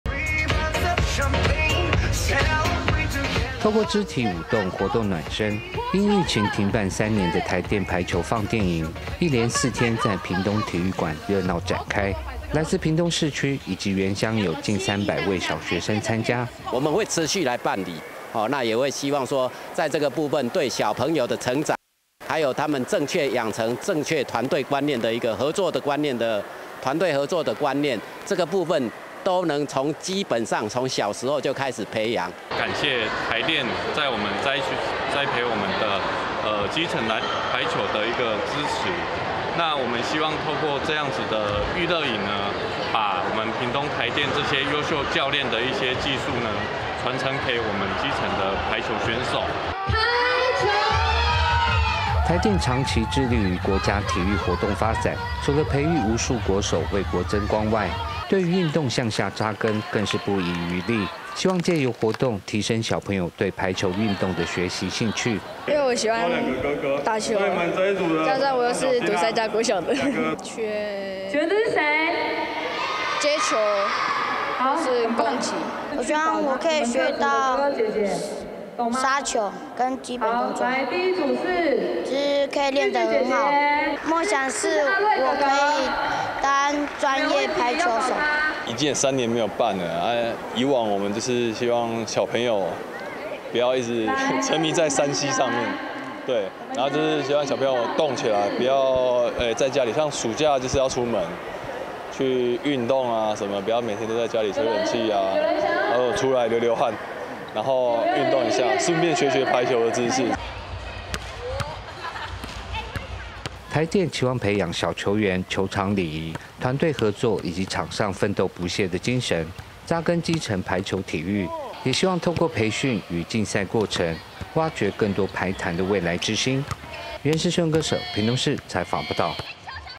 台電排球營前進屏東 扎根國中小基層體育｜每日熱點新聞｜原住民族電視台
台電 在暑假期間舉辦 排球Fun電營，提供學童學習排球運動的機會，屏東縣 共5所國中小學生參加，期盼藉由營隊扎根基層體育，培養小球員團隊精神、提升運動風氣。